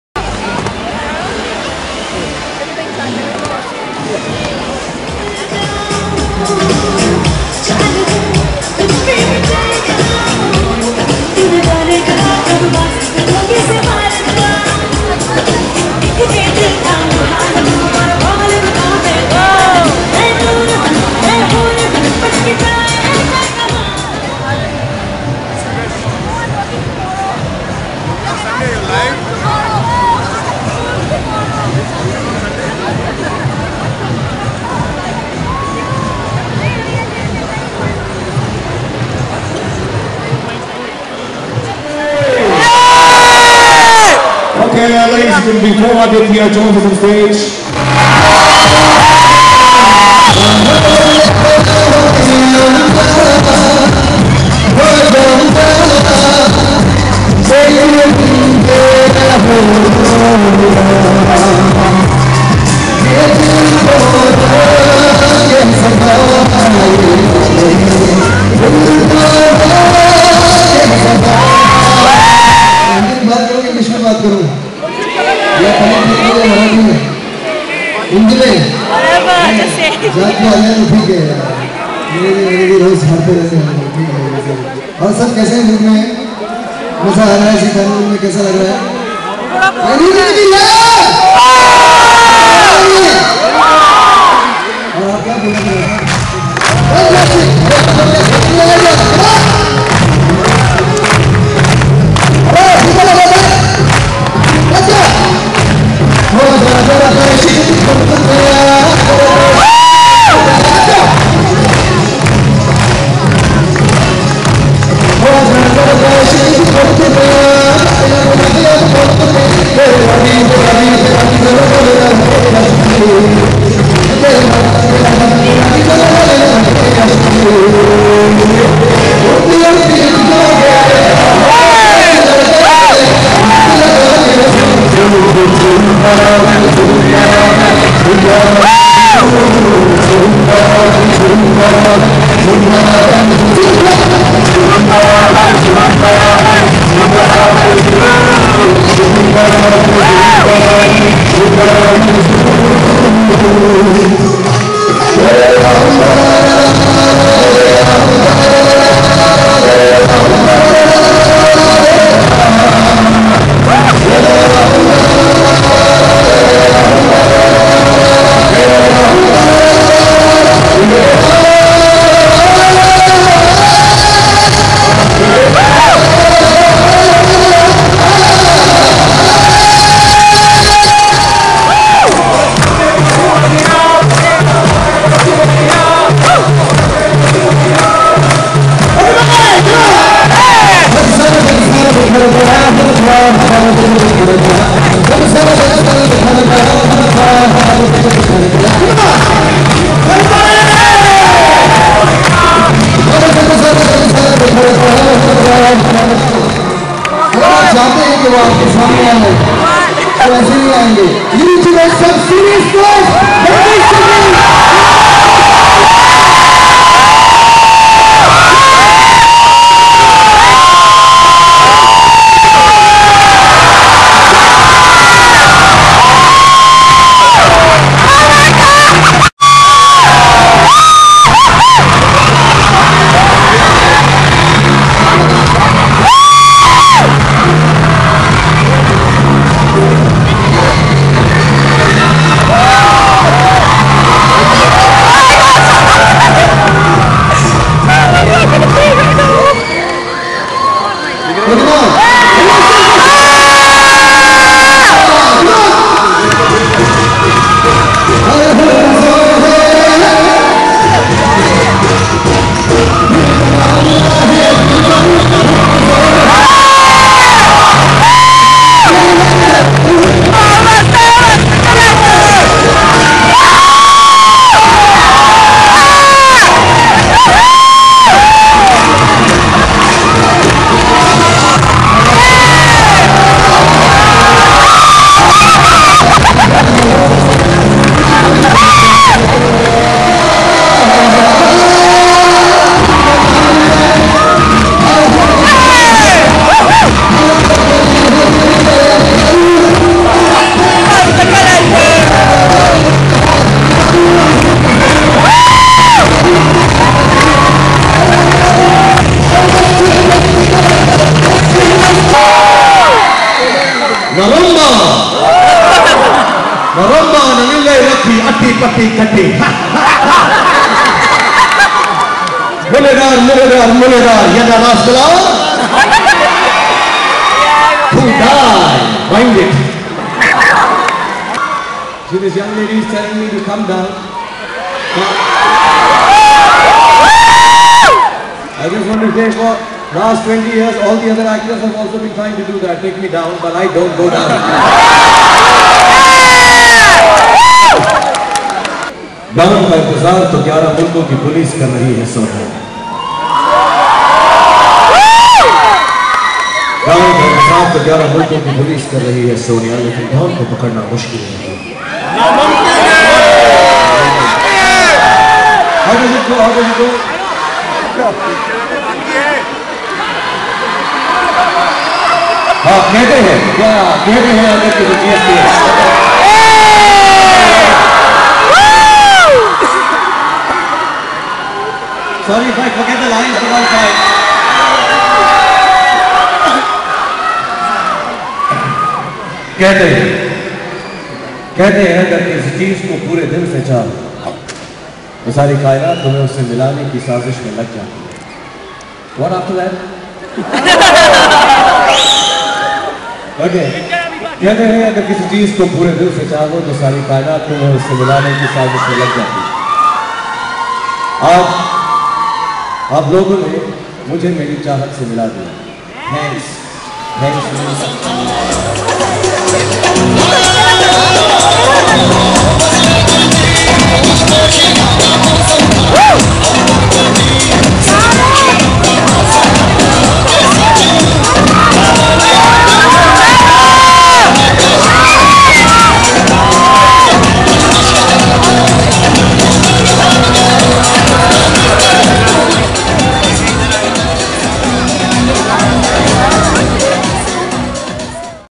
SHAH RUKH KHAN ZEE CARNIVAL 2008 SINGAPORE
Tinsel Gurus, met Shah Rukh khan, Aditya Narayan and Iqbal.